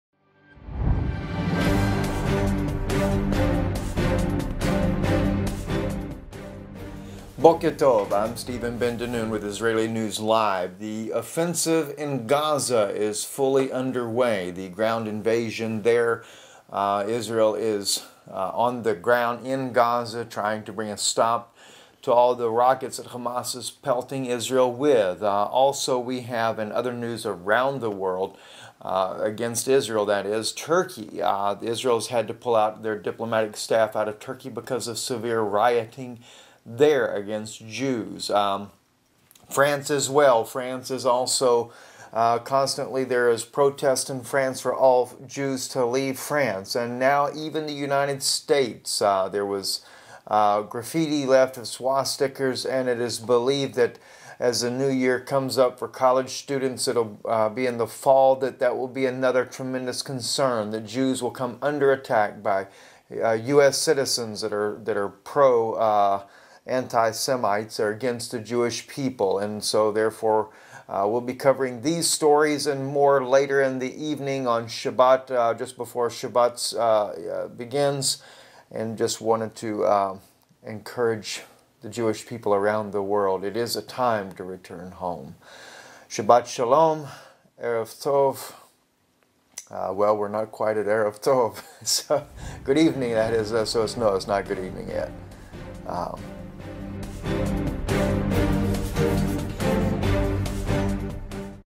Israeli News Live July 18th Update
Israel's ground offensive goes full swing to stop Hamas from its relentless assault against Israel. Rioting breaks out in Turkey against Jews